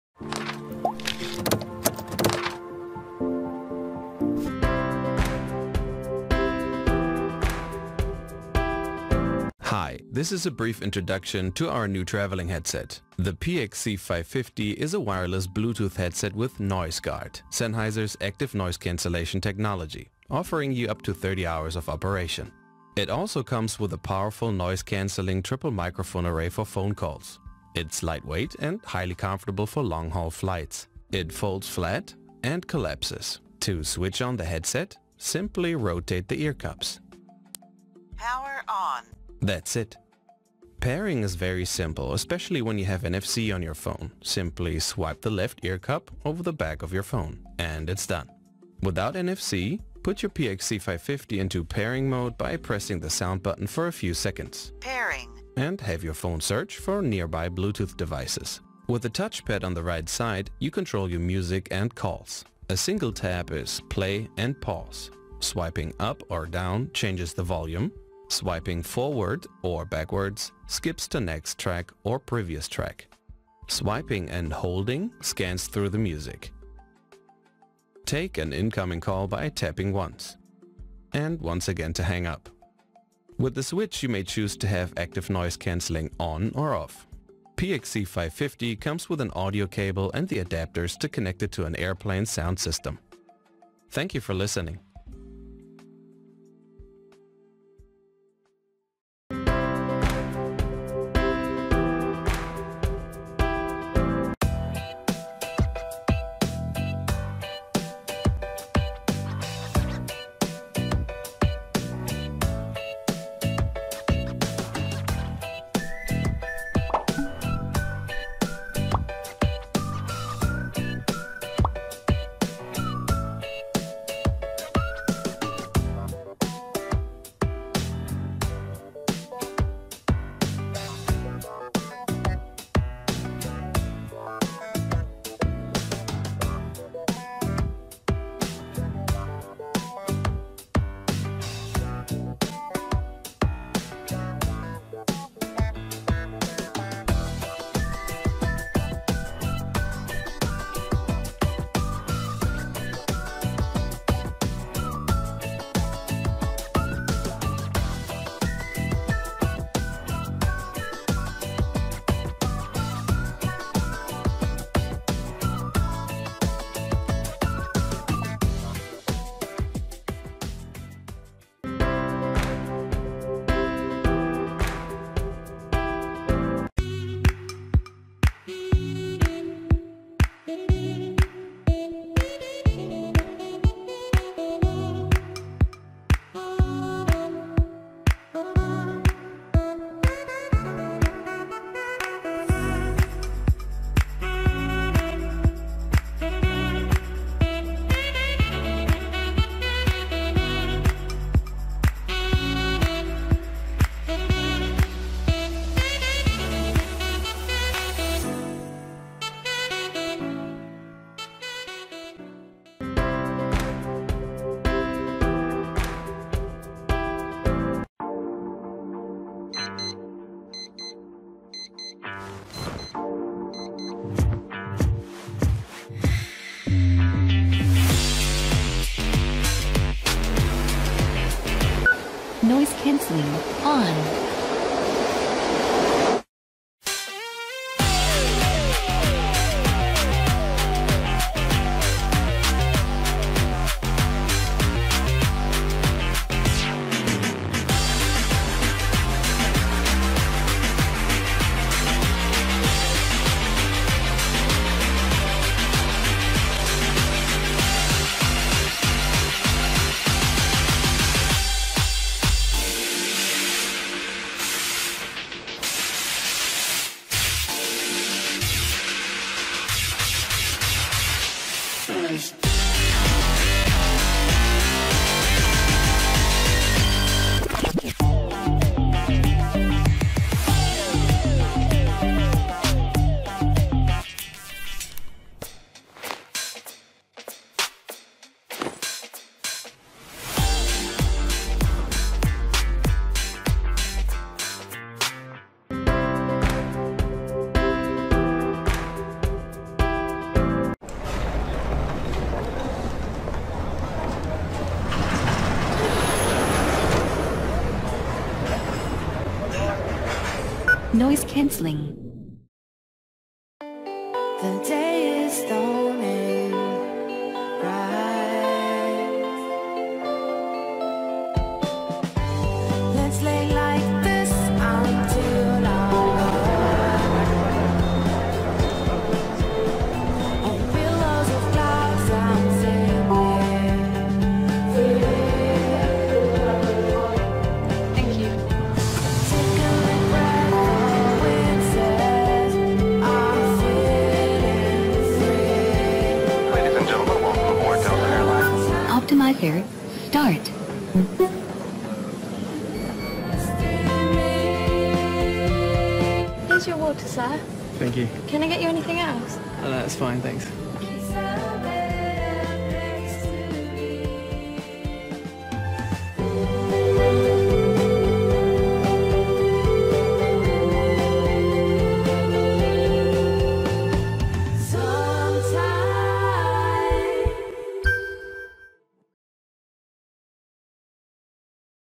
Top 5 Best Smart Headphones In 2020 - You Can Buy On Amazon
1. Sennheiser PXC 550 Wireless – NoiseGard Adaptive Noise Cancelling, Bluetooth Headphone with Touch Sensitive Control and 30-Hour Battery Life

2. 3D Sound Labs One - 3D Audio Headphones

3. Paww WaveSound 3 Bluetooth Headphones – Active Noise Cancelling Headphones with Airplane Adapter

4. Sony XB950N1 Extra Bass Wireless Noise Canceling Headphones

5. Sony Noise Cancelling Headphones WH1000XM2: Over Ear Wireless Bluetooth Headphones with Case

Today, we reviewed the 5 best Headphones  that you can buy this year. I made this list after testing many different Headphone and i ranked these based on their price, quality, durability and more.